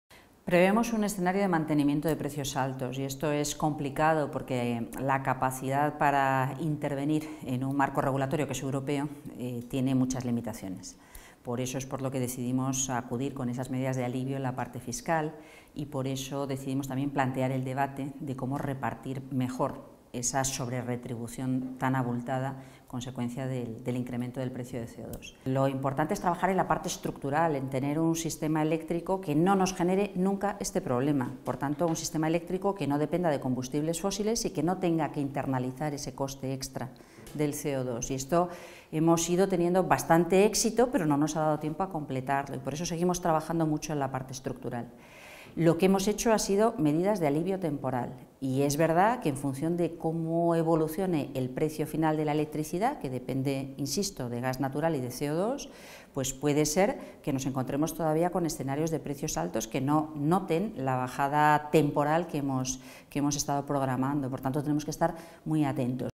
Ribera: la subida de la electricidad seguirá a pesar del alivio de los impuestos | Entrevista Ribera
infoLibre entrevista a la vicepresidenta cuarta y ministra de Transición Ecológica: "Pedimos una cierta contención a las eléctricas y que entiendan la posición del consumidor".